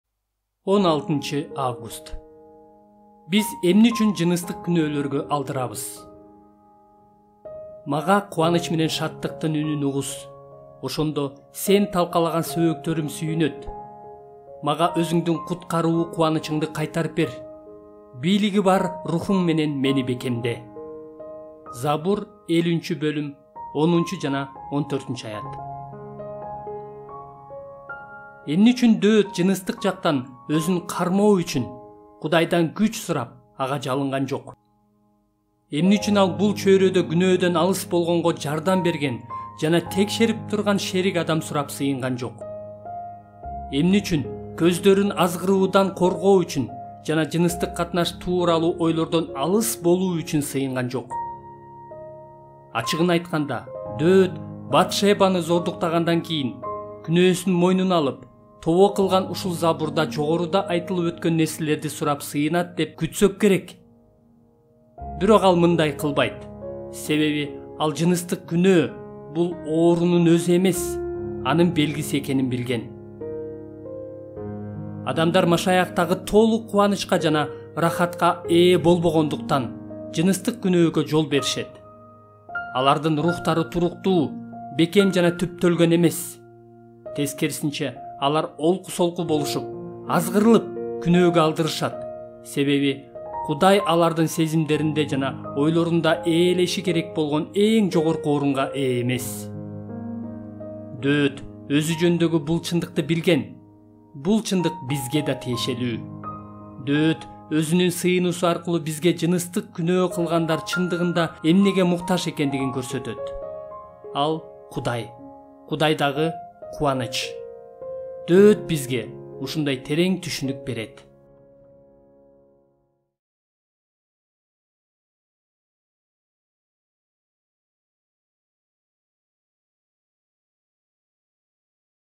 16-август: Биз эмне үчүн жыныстык күнɵɵлɵргɵ алдырабыз?
16-август
Сиз эмне үчүн жыныстык күнɵɵлɵргɵ азгырыласыз?
Жон Пайперден насаат сɵз

«Мага кубаныч менен шаттыктын үнүн угуз, ошондо Сен талкалаган сөөктөрүм сүйүнөт…. Мага Өзүңдүн куткаруу кубанычыңды кайтарып бер, бийлиги бар Рухуң менен мени бекемде» (Забур 50:10,14).

Дɵɵт эмне үчүн жыныстык кармануу үчүн жалынган жок? Эмне үчүн ɵзүнɵ жооптуу боло турган адам үчүн сыйынган жок? Ал эмне үчүн кɵздɵрүн азгыруудан коргоо үчүн жана жаныстык катнаш тууралуу ойлордон алыс болуу үчүн сыйынган жок? Бат-Шебага жакындагандан кийин, күнөөсүн мойнуна алып, тобо кылган ушул забурда, Дөөттɵн жогоруда айтылып ɵткɵн нерселерди сурап сыйынат деп күткɵн болсоңуз керек.
Анын андай кылбаганынын себеби ал жыныстык күнɵɵ – бул оорунун ɵзү эмес, анын белгиси экенин билген.
Адамдар Машаяктагы толук кубанычка жана ырахатка ээ болбогондуктан жыныстык күнɵɵгɵ жол беришет. Алардын рухтары туруктуу, бекем болуп, түптɵлгɵн эмес. Тескерисинче, олку-солку болушуп, азгырылып, күнɵɵгɵ жол беришет, анткени Кудай алардын сезимдеринде жана ойлорунда ээлеш керек болгон эң жогорку орунга ээ эмес.
Дɵɵт ɵзү жɵнүндɵгү бул чындыкты билген. Бул чындык бизге да тиешелүү. Дɵɵт, бизге ɵзүнүн сыйынуусу аркылуу, жыныстык күнөө кылгандардын чыныгы муктаждыгы: Кудай! Кудайдан кубаныч алуу экенин көрсөтүп жатат.
Бул биз үчүн терең акылмандуулук болуп саналат.